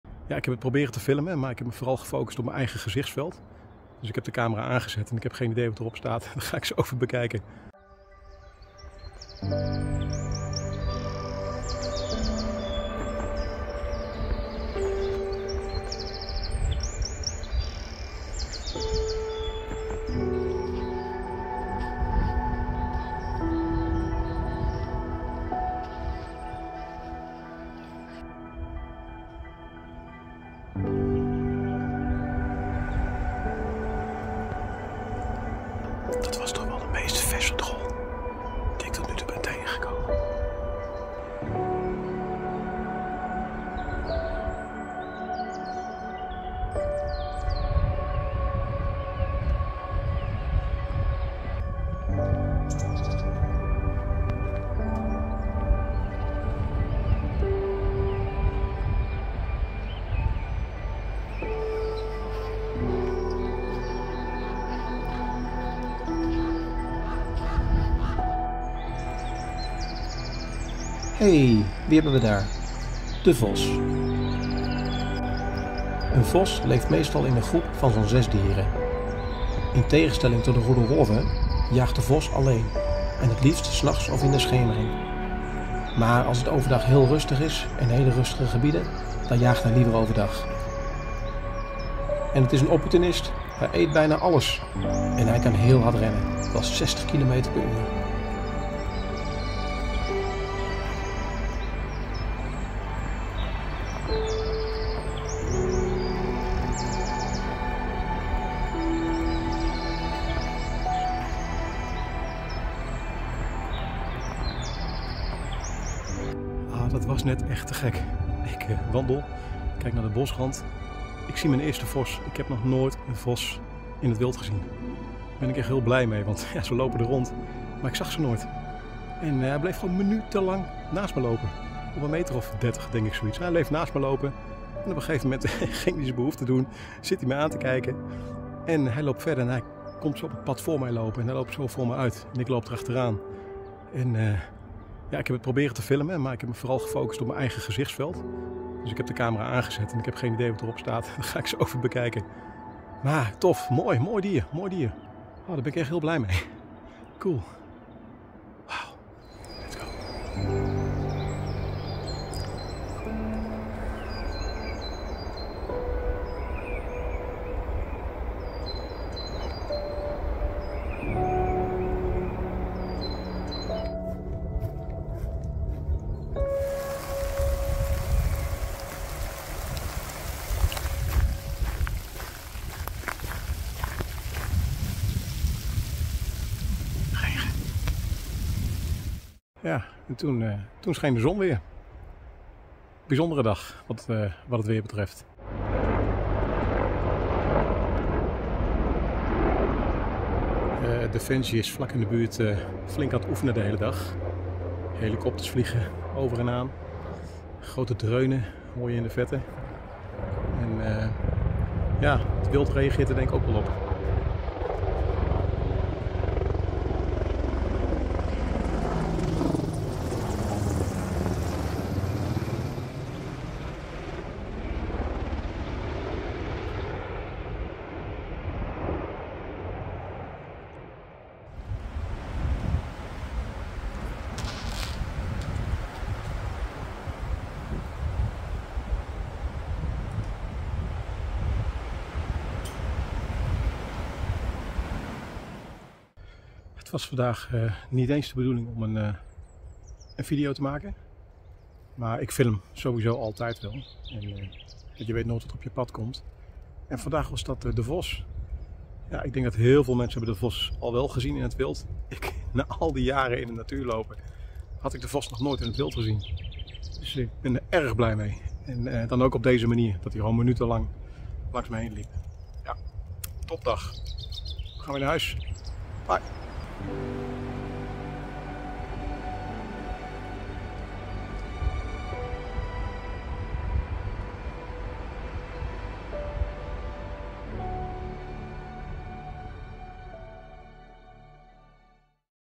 #19 - EEN SLUWE ONTMOETING
Al jaren loop ik al te kuieren door de bossen. Van alles gezien, de Das, Wolf, maar nog nooit een Vos, in tegenstelling tot veel anderen. Ik kon tijdens deze wandeling mijn geluk niet op, het mooie dier liep minutenlang mee aan mijn zijde.